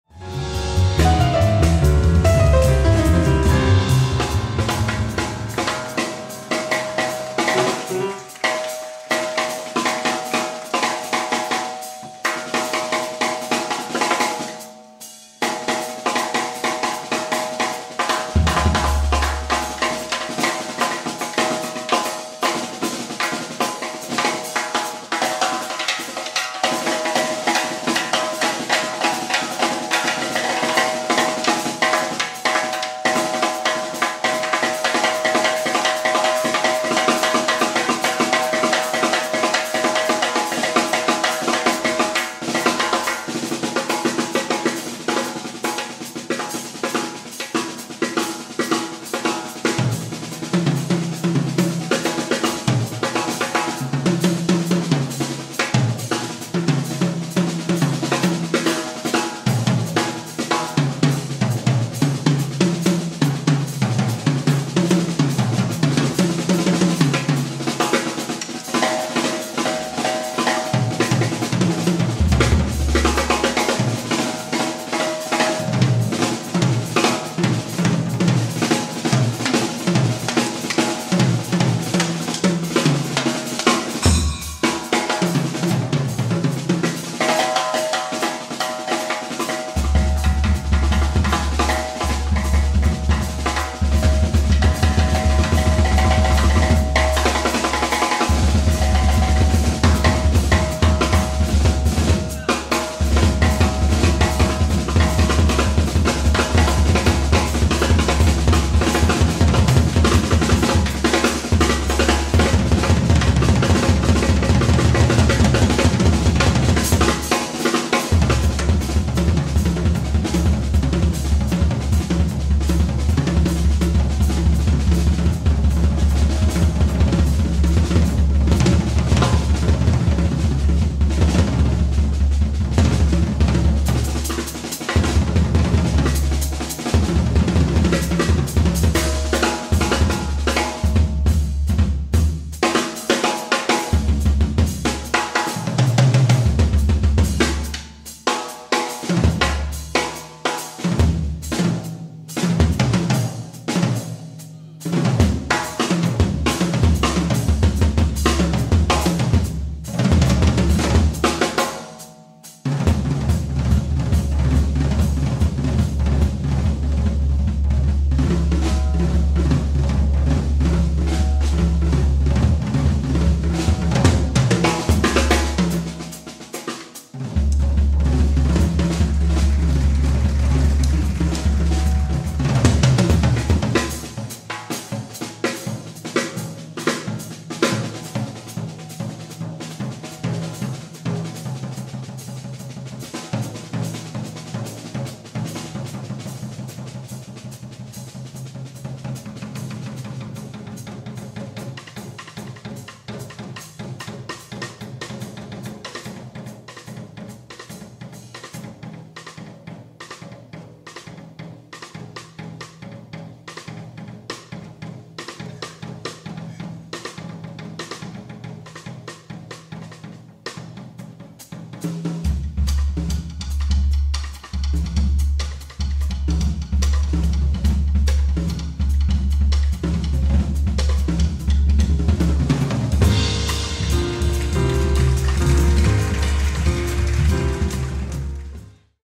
Kristijan Krajnčan - OPEN DRUM SOLO
Courtesy of Salihara, Indonesia.

Sri Hanuraga - piano, Mattia Magatelli - bass, Kristijan Krajncan - drums